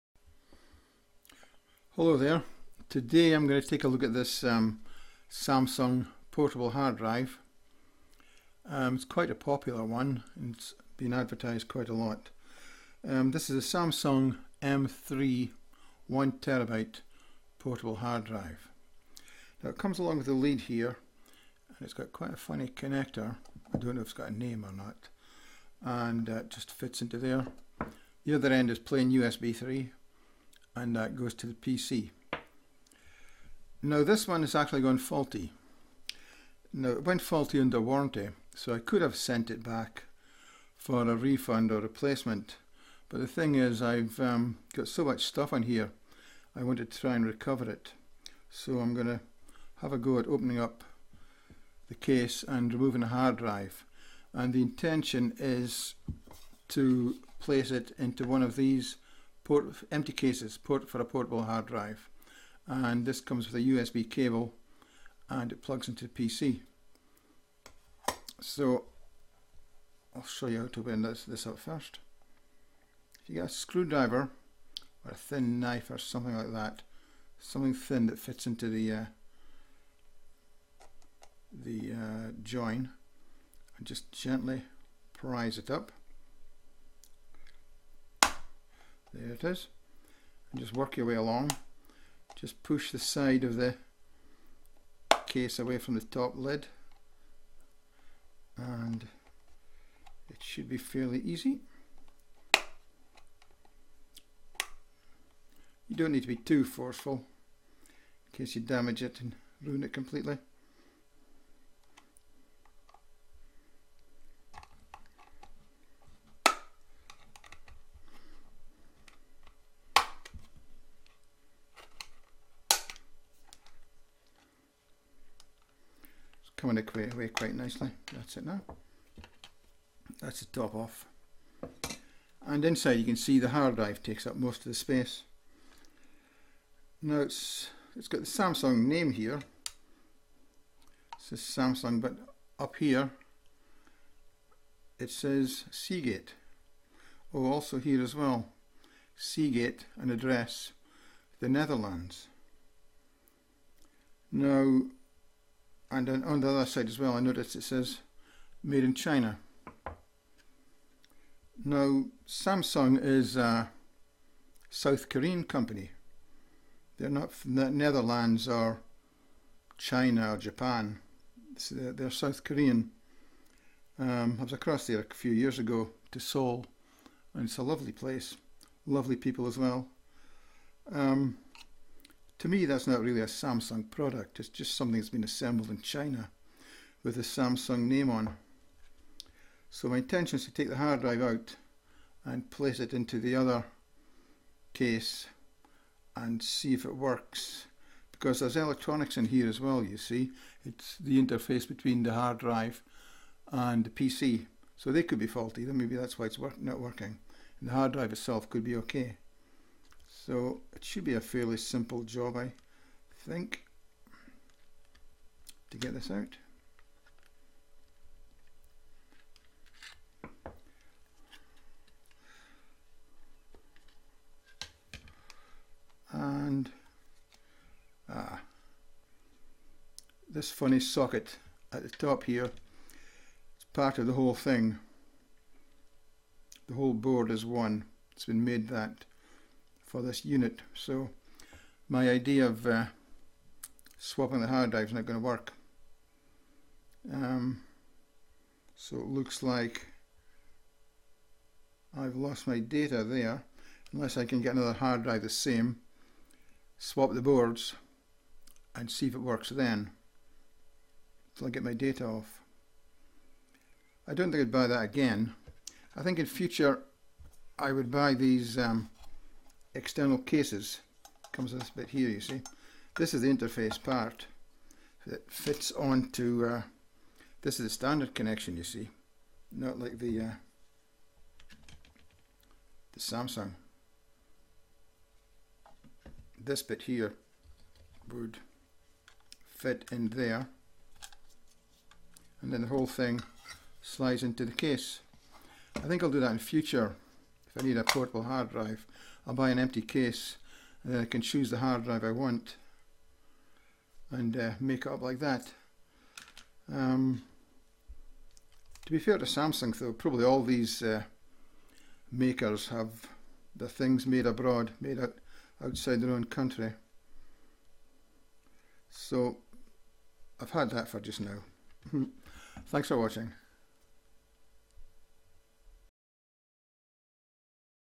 Inside the Samsung M3 1Tb Portable Hard Drive Review
If you enjoyed this, consider buying me a coffee. Thanks.
The Samsung M3 1Tb Portable Hard Drive has sold in large numbers.
How reliable are they, and what's inside?